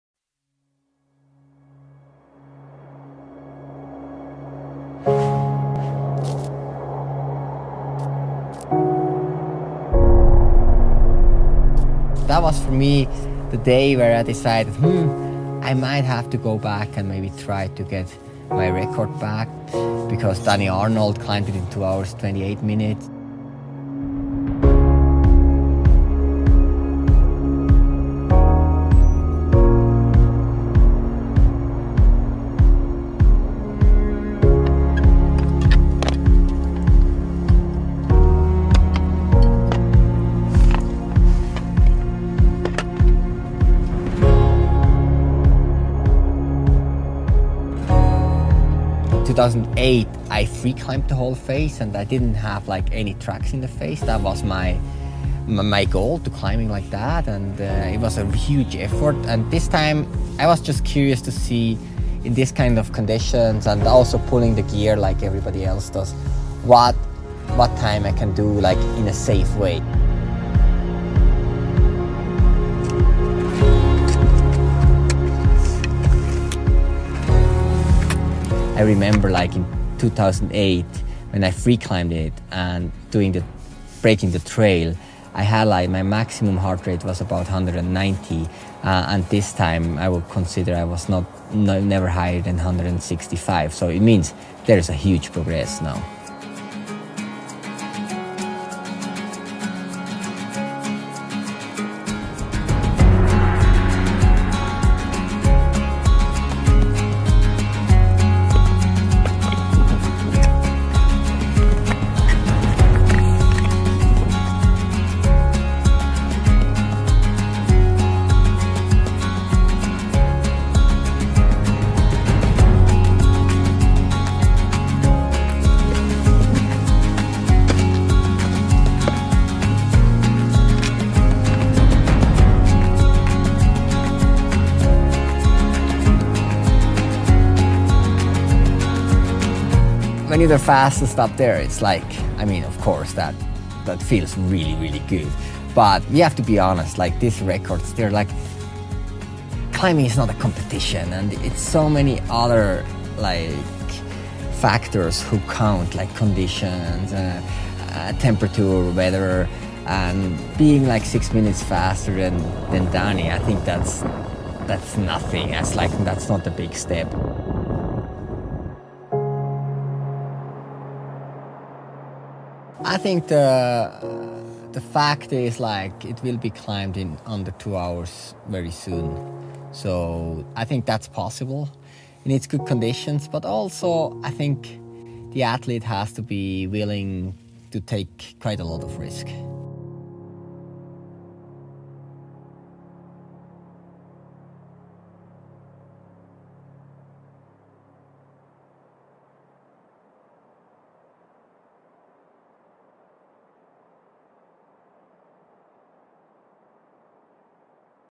Ueli Steck, Nuevo record Eiger: 2015
NUEVO RECORD EIGER:  2015
8 dias despues de ascender el Eiger con Kilian Jornet, y tan solo 5 dias después de establecer un record de cordada junto a Nicolas Hojac (3h 46min),  Ueli steck volvía a enfrentarse en solitario a la pared que ya ha ascendido en 40 ocasiones.
El resultado: Nuevo record del Eiger, situando el crono en 2h 22min 50s.  Ueli  recupera así el record que le había arrebatado en 2011 Dani Arnold (2h 28min).
 Ueli reconoce que, a diferencia de su ascensión del 2008, en la que subió en libre, tenía buena huella y ha usado el material que había fijo en el recorrido, el mismo estilo que usó Dani Arnold.
En 2008 sus pulsaciones se situaron en 190, mientras que para este nuevo record no ha necesitado sobrepasar las 160.